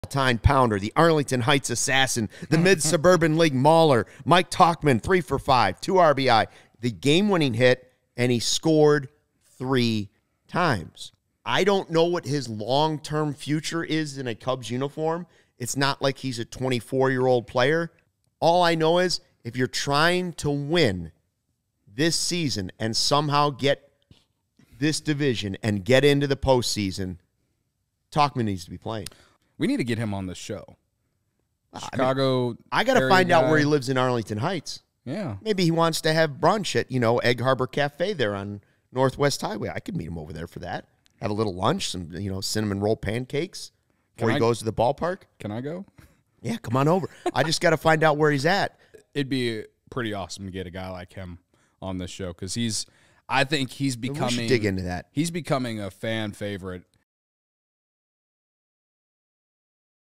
Mike Tauchman is the Chicago Cubs’ hometown hero!
Mike Tauchman is a 32-year-old Chicagoland native who has been a spark for the Chicago Cubs lately. Luke Stuckmeyer and Cody Delmendo talk about his impact and whether or not he'd be up for a podcast visit this season.